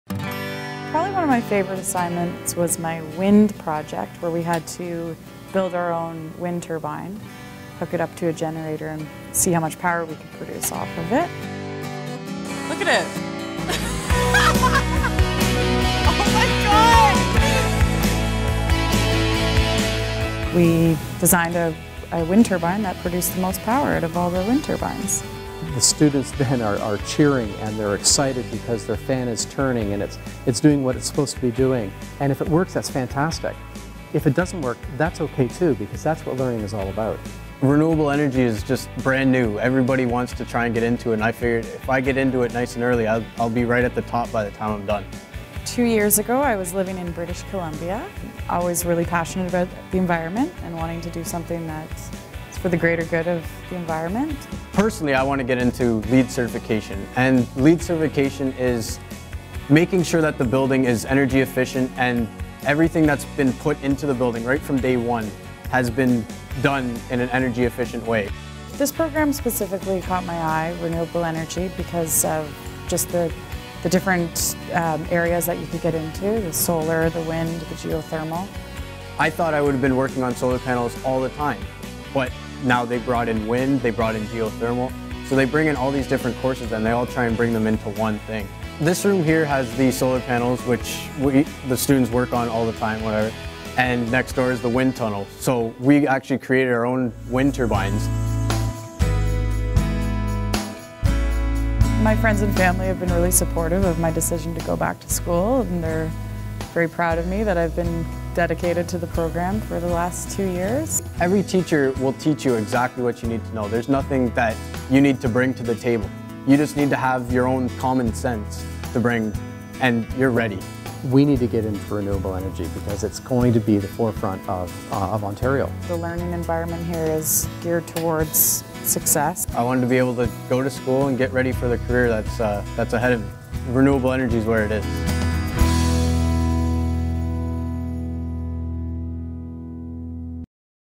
Renewable Energy Technician - Durham College
Renewable energy technicians are part of the rapidly growing field of renewable, sustainable and alternative energy solutions. You will receive a basic exposure to traditional energy consuming systems followed by a solid foundation in the operation of renewable energy systems including solar and wind power and ground source heat pumps.

As a renewable energy technician, you will be well positioned to pursue an interesting and challenging career in the energy sector.

This program is designed to meet the growing demand for technicians with the knowledge, skills and ability to perform energy audits on new and existing homes and assist with the installation and assembly of solar energy panels, geothermal heat pump systems and wind turbines.